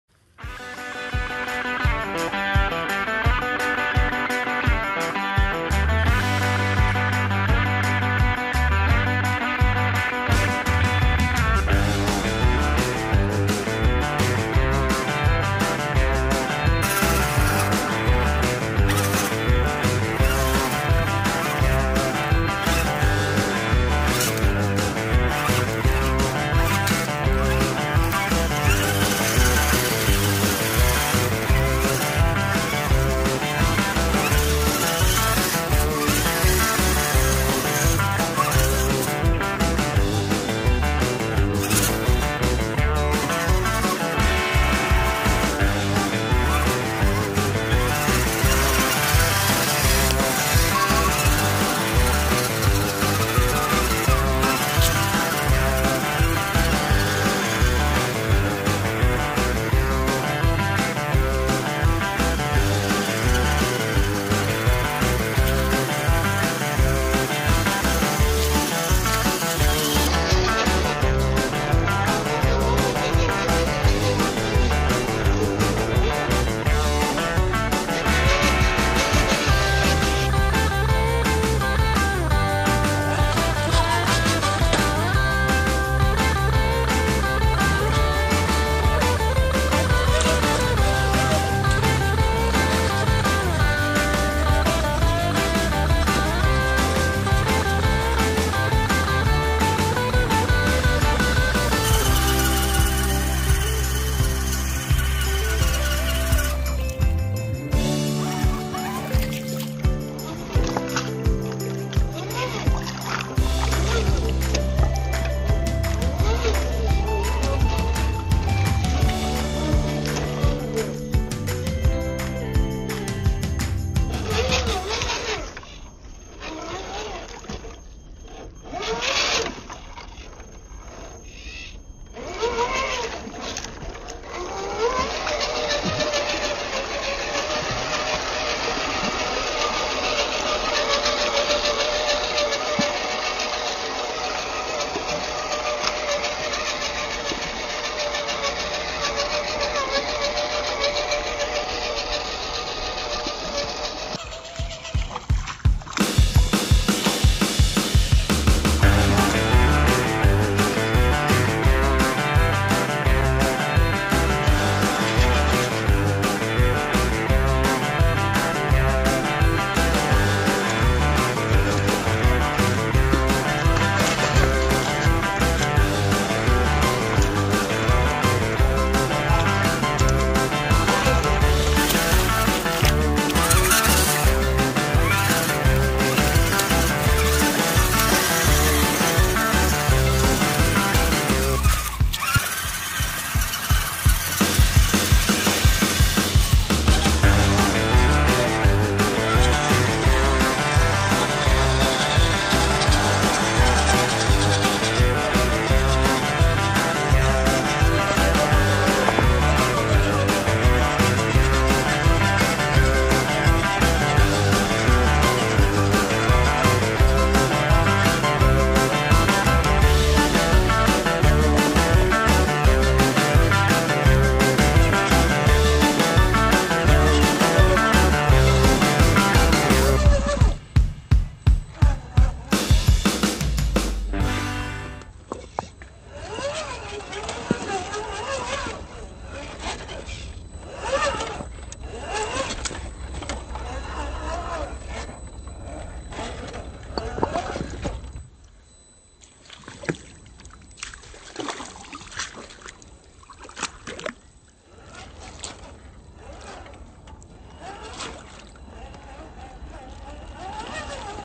Platform Swamp Buggy 1st Run Everest 10 Mudd Bogging with the WWE Team
Still have a lot of tweaking to do
the servos from Redcat are weak but at there prices it's not a big deal. and the shocks I used were to weak. for the money this is a fun truck no complaining. there's allot of different projects I can do wit the Everest 10 platform thinking of doing a 6x6 or even a 8x8 since I bought 2. wish some of the other RC truck companies with get real with there pricing, there just too expensive like Axial way overpriced for the lack of quality. anyway fun truck.